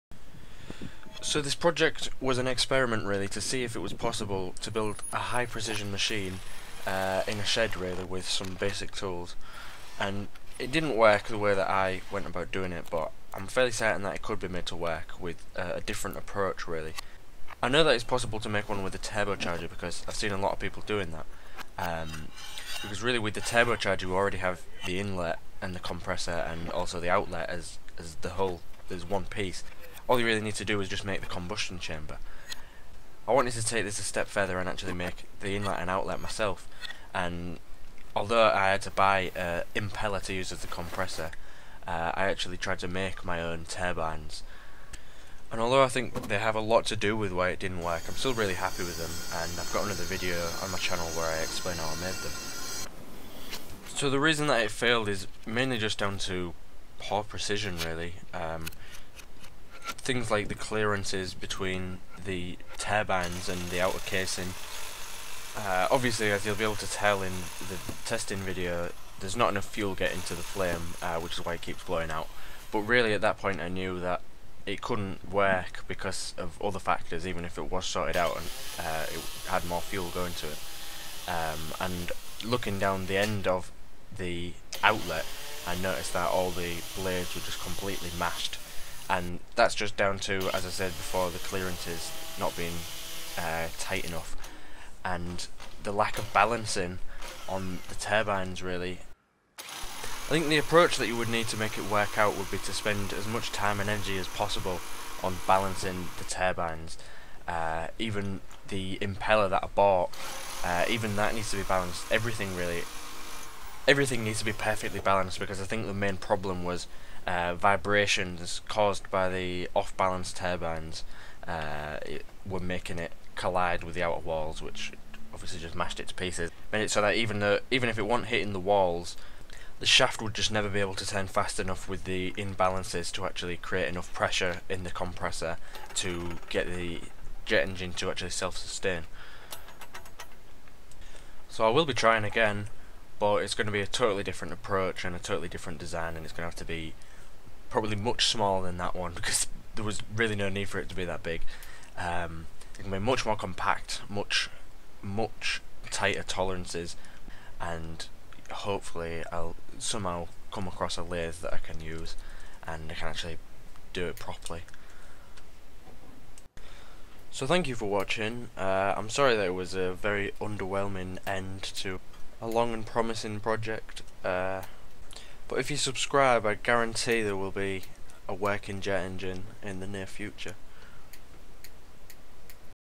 Homemade Turbojet Engine Failure and Review
Sadly my turbojet engine project has been a resounding failure. This video shows the assembly, testing footage and I talk about why it failed.

🔌 Power Supply
🔥 Soldering Station